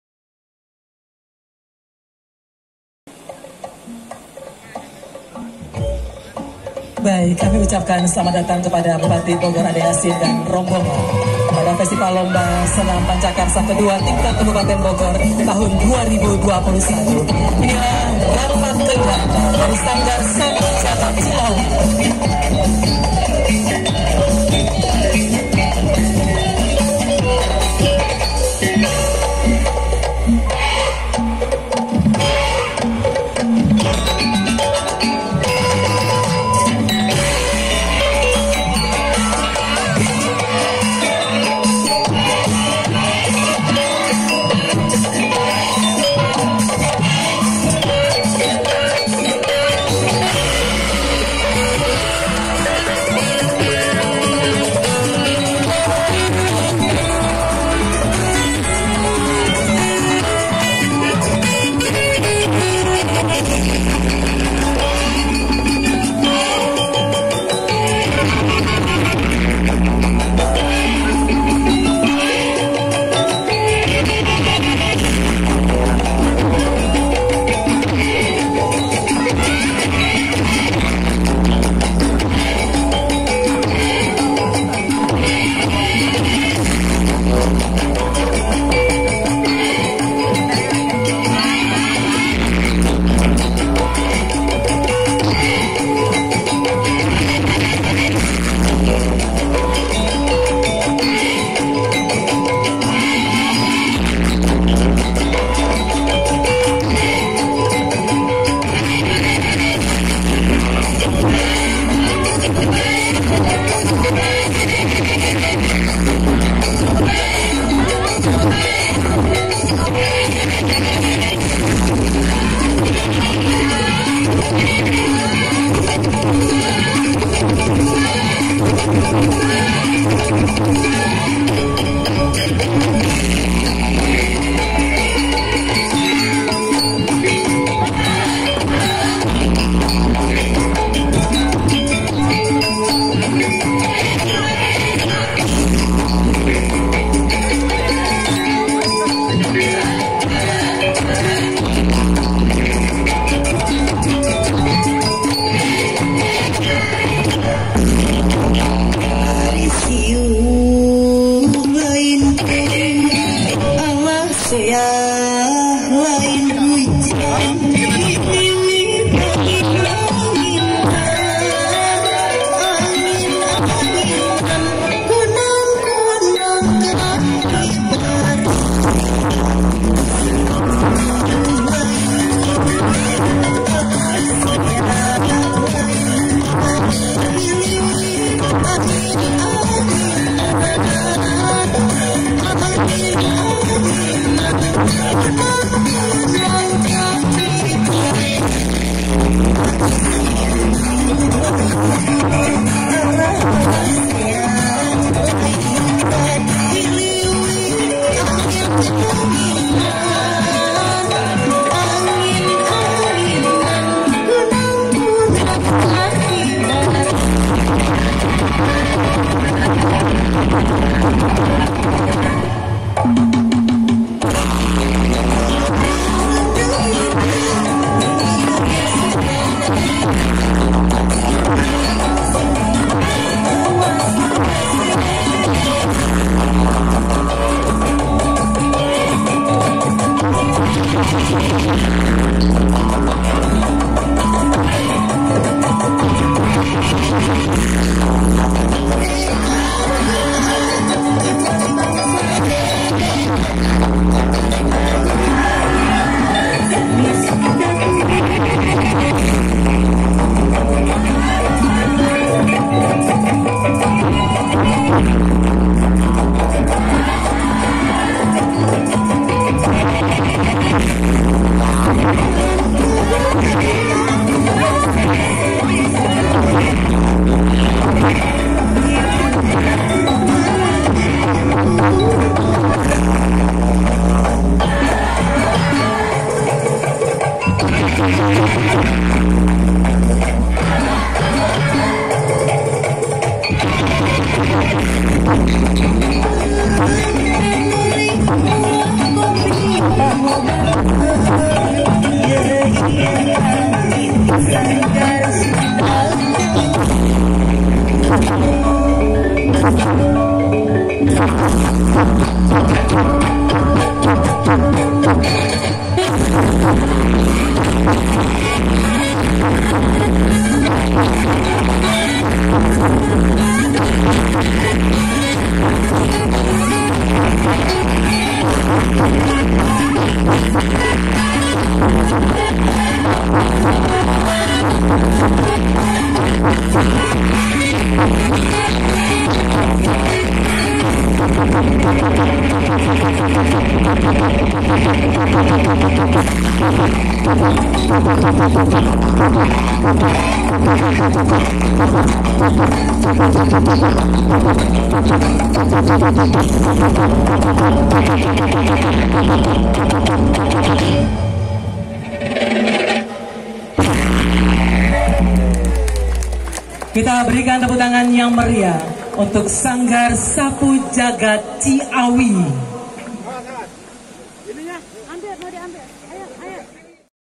Pembukaan Final Senam Pancakarsa oleh Sanggar Sapujagat Ciawi Part 1 -Berbagi Media
Pembukaan Final Lomba Senam Pancakarsa oleh Sanggar Sapujagat Ciawi
yang diselenggarakan PORMI, KORMI dan Pemerintah Kab. Bogor
di Stadion Pakansari